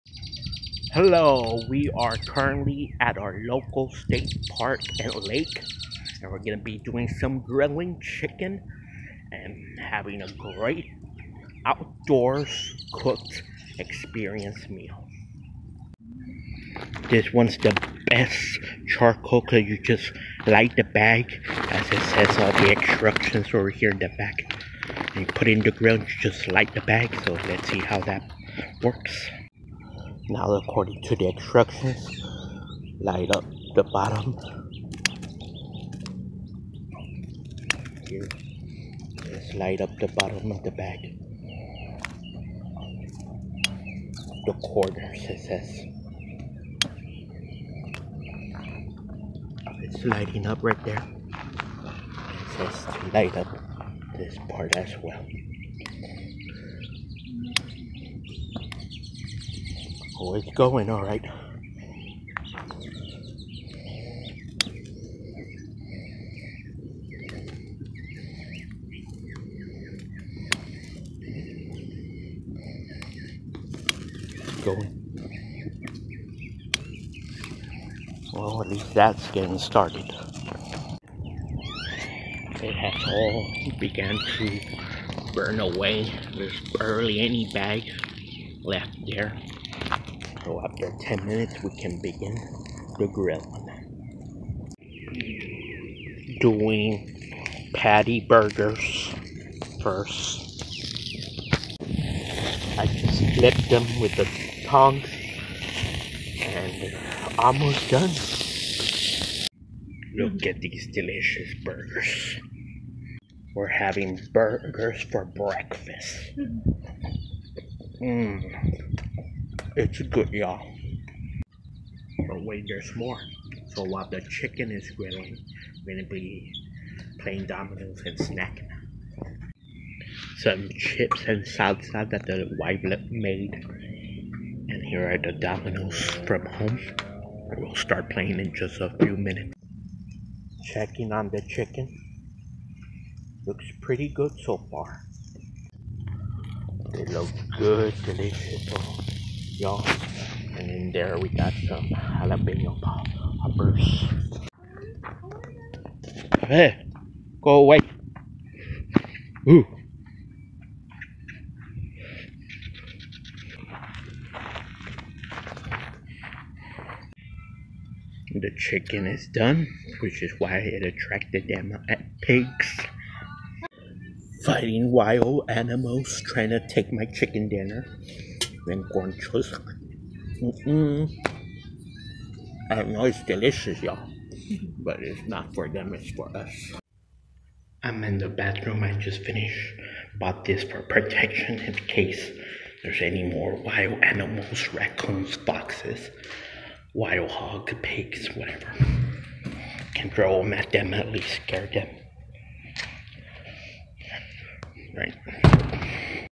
Wild Hogs Disrupted Our Family Cook Out At The Lake | Grilling Burgers and Chicken
We cook burgers and chicken at the lake, wild Hogs attacked.